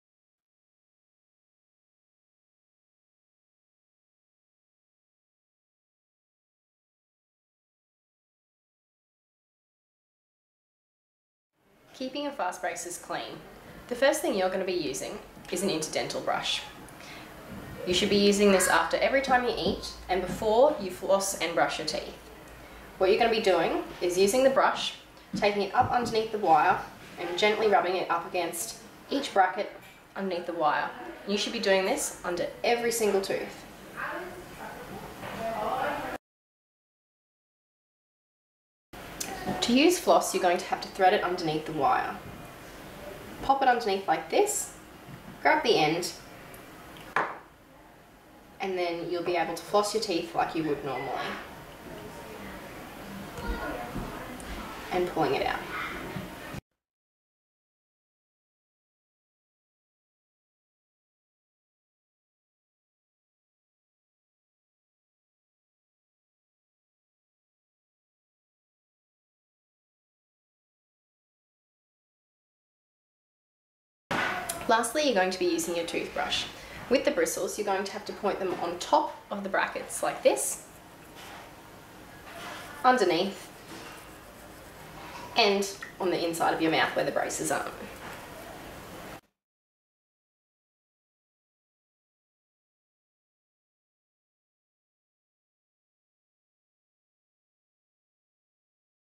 Cleaning Fastbraces
It's very important to clean around every bracket and in between every single tooth to maintain optimal dental health. We will provide you with wax, floss, a proxy brush and a toothbrush.